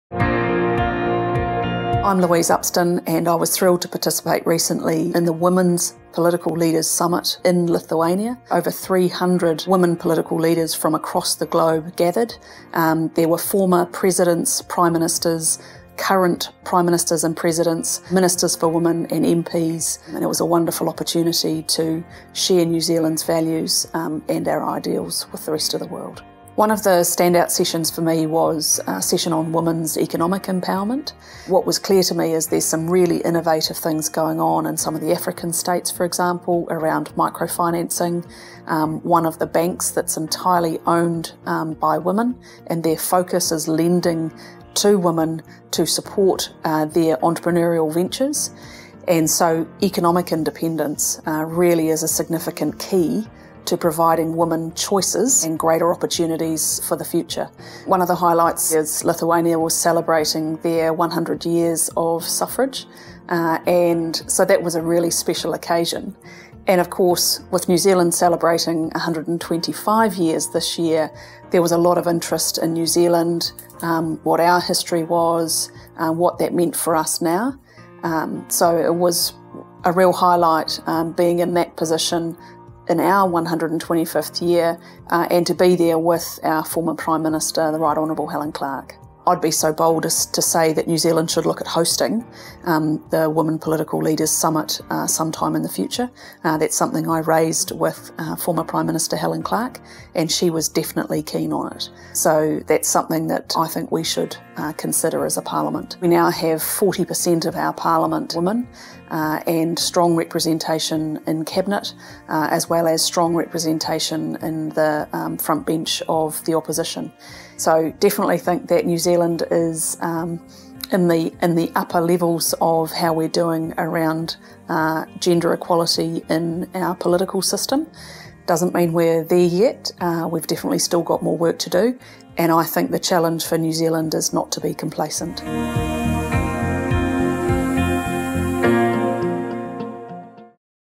Women Political Leaders Global Summit 2018: Hon Louise Upston MP
In June 2018, New Zealand MP Hon Louise Upston joined more than 250 women leaders from around the world at the Women Political Leaders Global Summit in Vilnius, Lithuania. We caught up with her to hear more about the experience.

The keynote address, ‘It’s about time! Women change the World’ was given by the President of the Republic of Lithuania, Dalia Grybauskaite. Speakers included Hillary Clinton, Antonio Guterres (the Secretary-General of the United Nations), Prime Minister of Iceland Katrin Jakobsdottir, and NZ's very own Rt Hon Helen Clark.